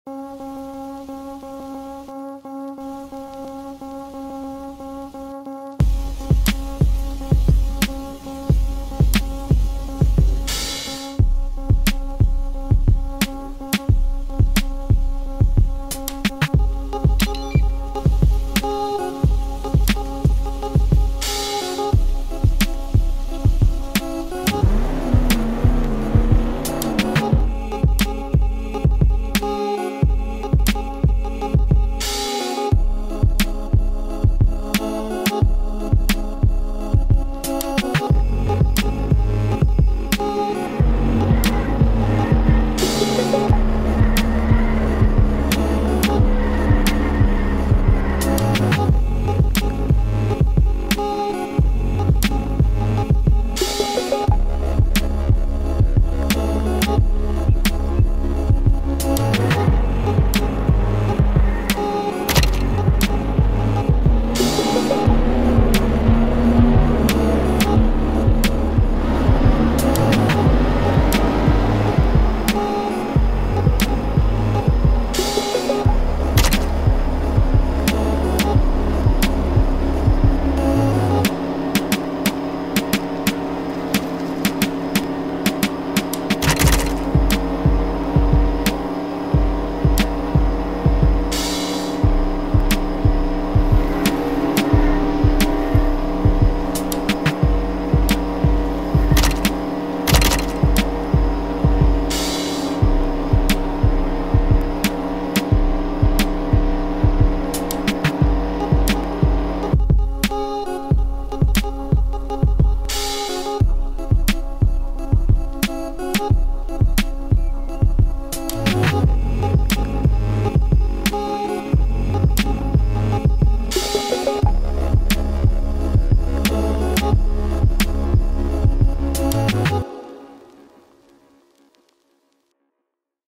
the new limited countach (torero) supercar review
throwback lamborghini B)
super cool car with limited availability